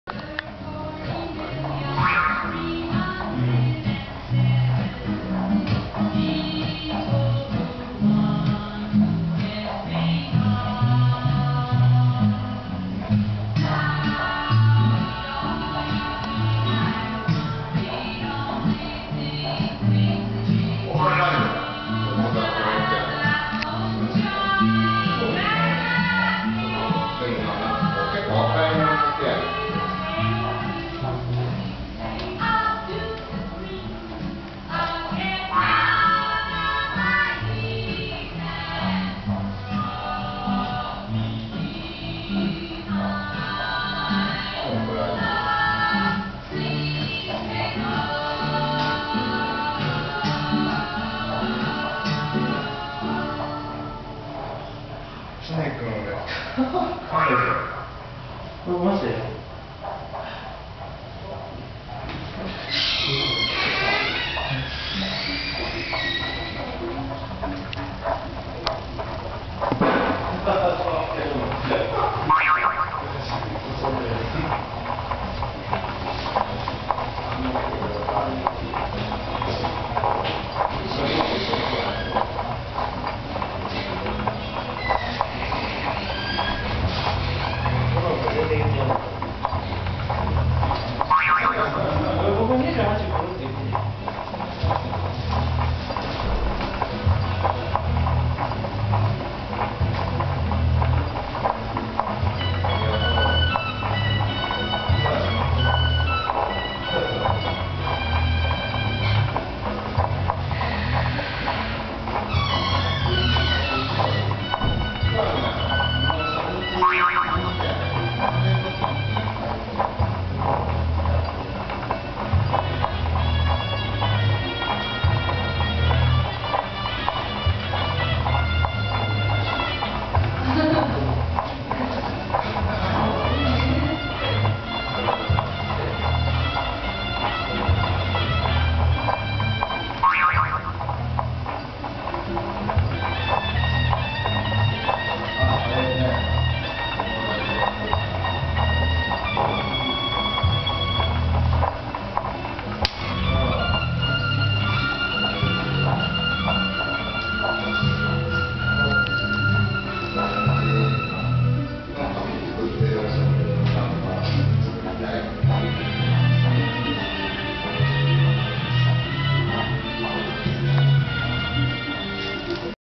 exhibition [CHAT]
[CHAT]exhibition @FUKUGAN GALLERY. Artists are Atsuo Nakagawa & Mimi Murai / Naohiro Gonda.